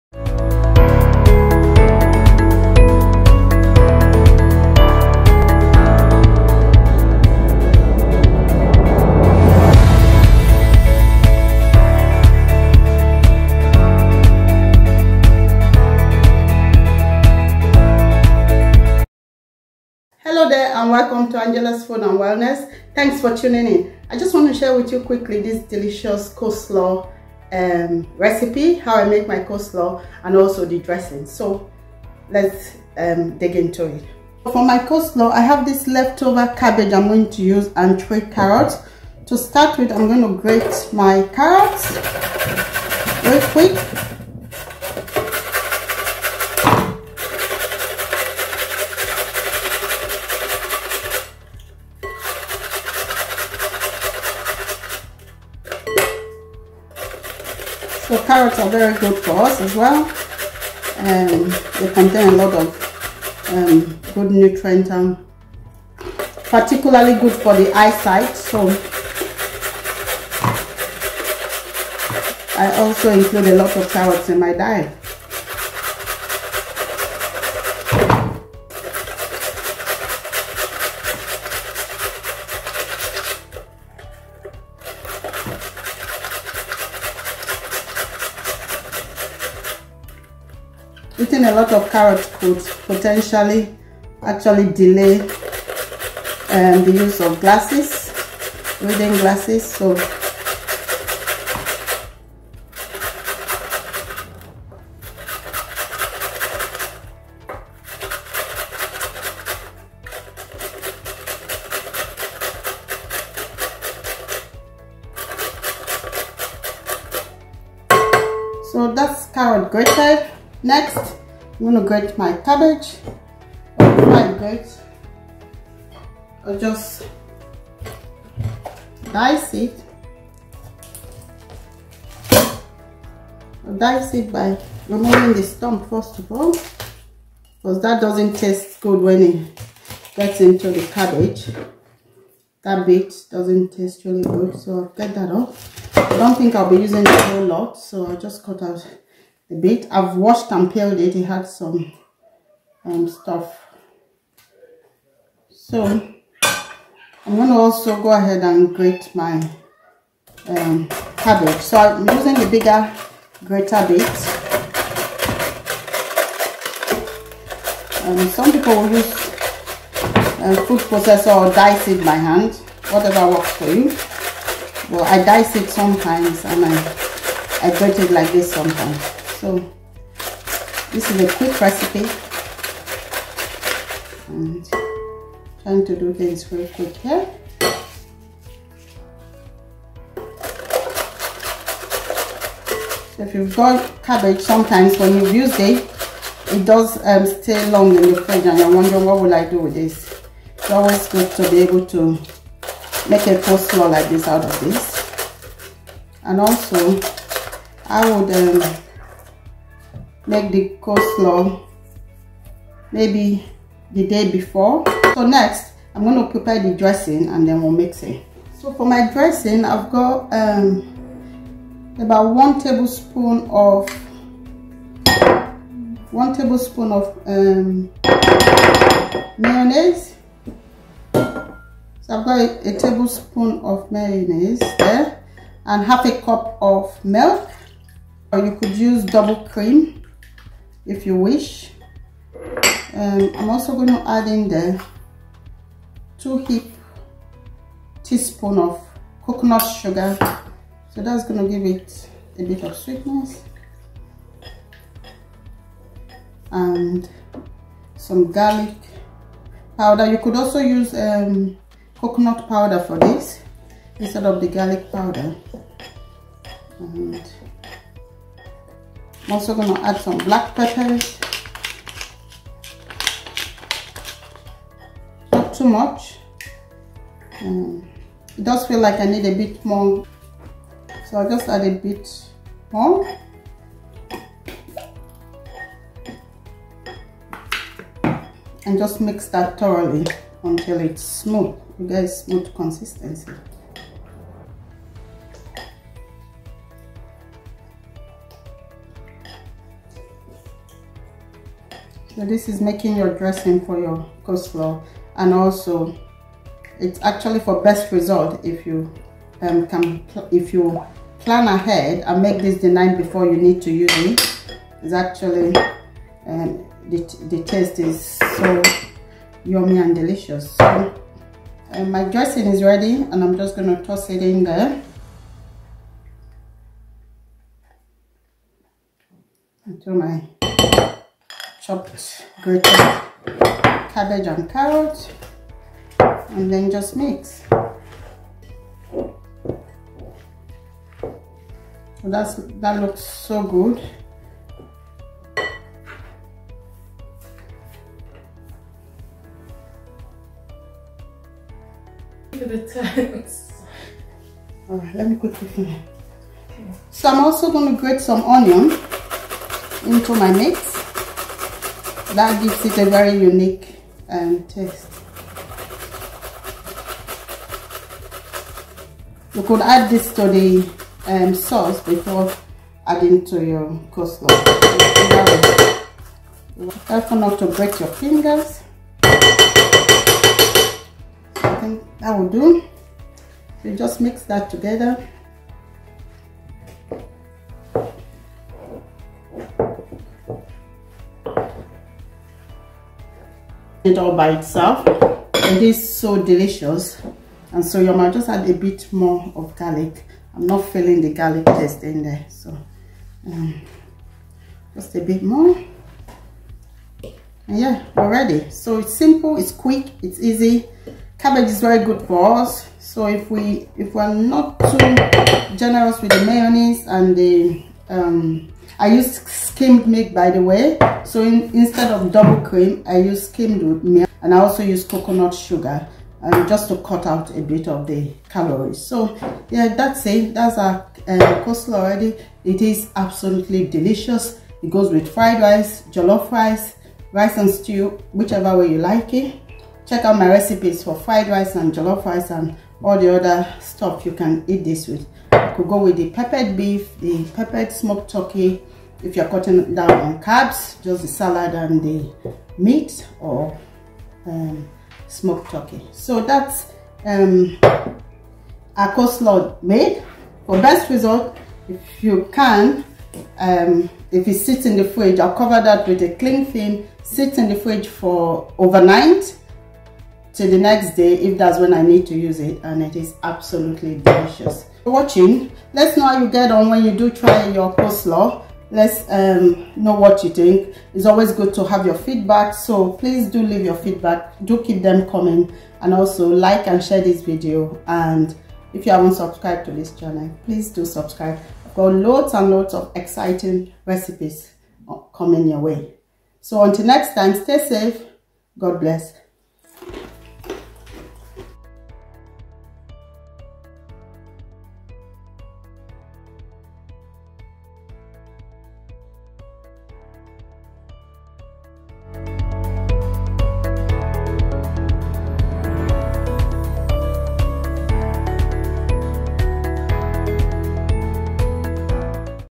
Healthy Homemade Coleslaw
Ingredients
1/4 Cabbage (shredded)
3 peeled carrot (grated)
2 tsp coconut sugar
1 tbsp mayonaise
1/2 cup skimmed milk
1/2 tsp garlic powder
1/2 tsp grated onions